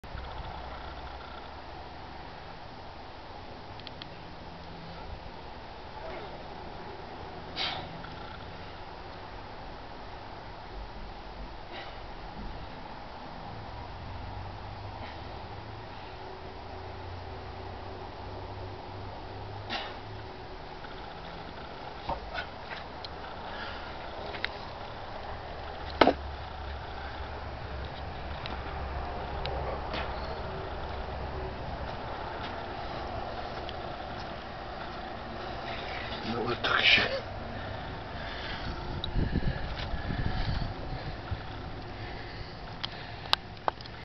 ПОДЪЁМ И ЖИМ ГАНТЕЛИ 50 КГ С РУЧКОЙ 75 ММ ОДНОЙ РУКОЙ.avi
Подъём на грудь и жим гантели 50 кг с толстой ручкой 75 мм одной рукой, 50kg dumbbell with thick handle 75 mm one hand clean and press